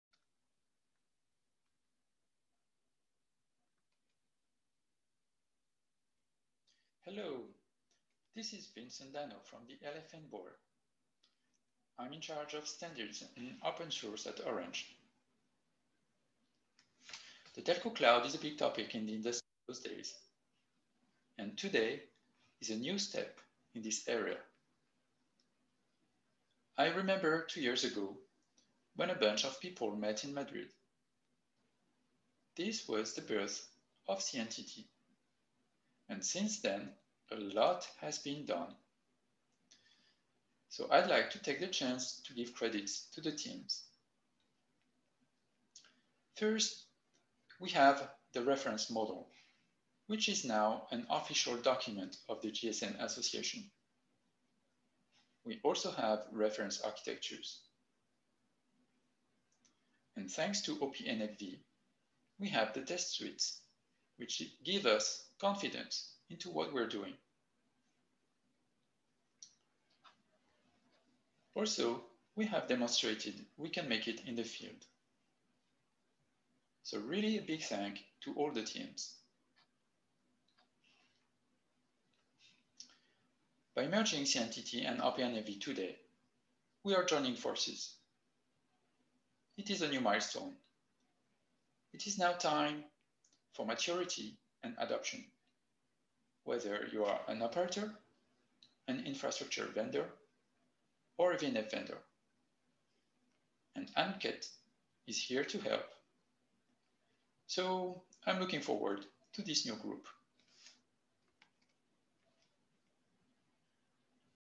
Anuket Launch: Message from LFN Board: Vincent Danno
Anuket delivers a common model, standardized reference infrastructure specifications, and conformance and performance frameworks for virtualized and containerized network functions, enabling faster, more robust onboarding into production, reducing costs and accelerating communications digital transformations. In this industry leadership spotlight video, hear from LFN Board Member, Vincent Danno, Orange.